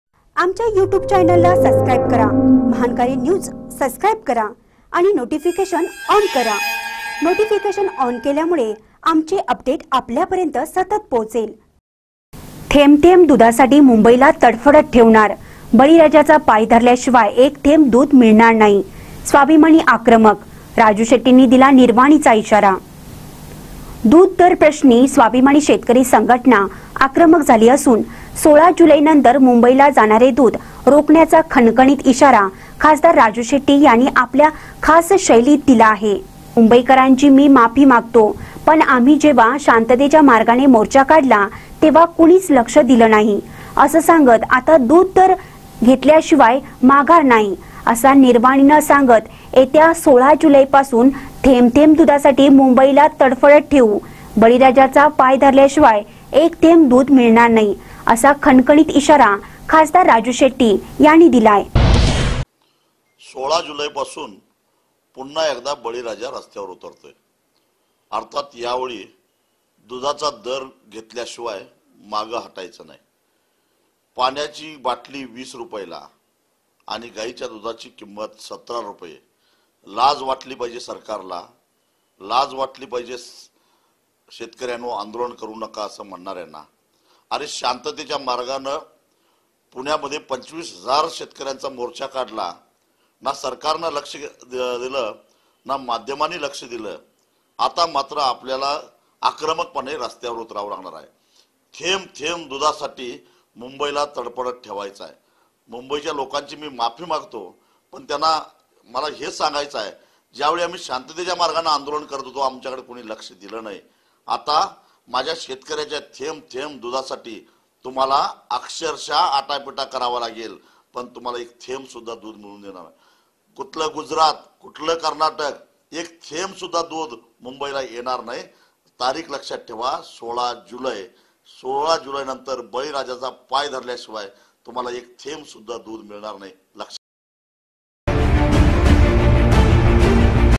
थेंब थेंब दुधासाठी मुंबईला तडफडत ठेवणार : बळीराजाचा पाय धरल्याशिवाय एक थेंब दूध मिळणार नाही
थेंब थेंब दुधासाठी मुंबईला तडफडत ठेवणार : बळीराजाचा पाय धरल्याशिवाय एक थेंब दूध मिळणार नाही- स्वाभिमानी आक्रमक, राजू शेट्टींनी दिला निर्वाणीचा इशारा